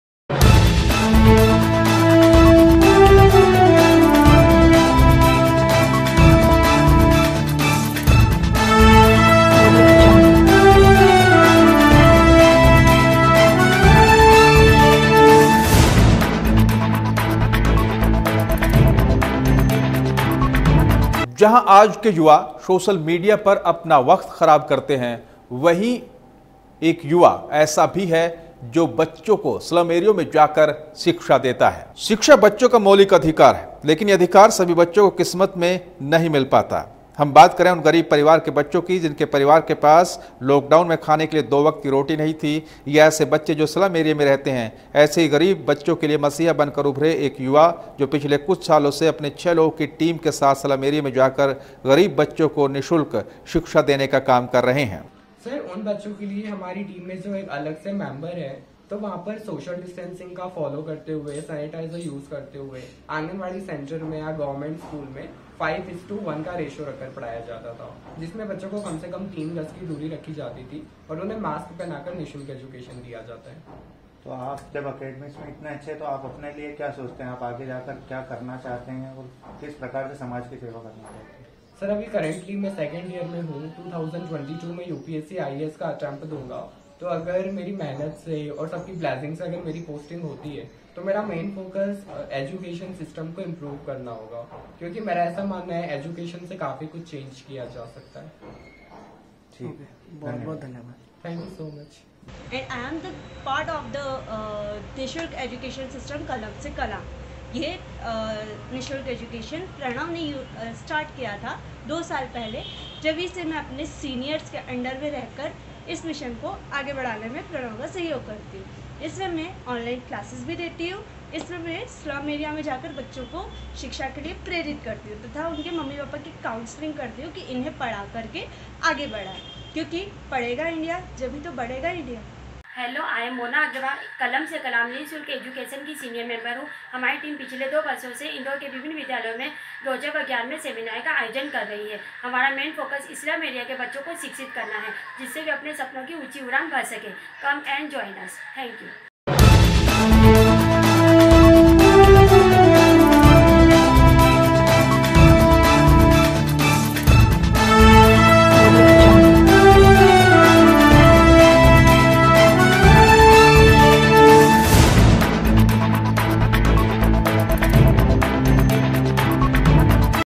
19 वर्ष के प्रणय गरीब बच्चो को स्लम एरिया में जाकर देते बच्चो को निःशुल्क शिक्षा
शिक्षा बच्चों का मौलिक अधिकार होता है
लेकिन यह अधिकार सभी बच्चों की किस्मत में कहां. हम बात कर रहे हैं उन गरीब परिवार के बच्चों की जिनके परिवार के पास इस लॉकडॉउन में खाने के लिए दो वक्त की रोटी नहीं थी या ऐसे बच्चे जो स्लम एरिया में रहते हैं ऐसे ही गरीब बच्चो के लिए मसीहा बनकर उभरे एक युवा जो पिछले कुछ वर्षों से अपनी 6 लोगो की टीम के साथ स्लम एरिया में जाकर गरीब बच्चों को निशुल्क शिक्षा देने का काम कर रहे हैं।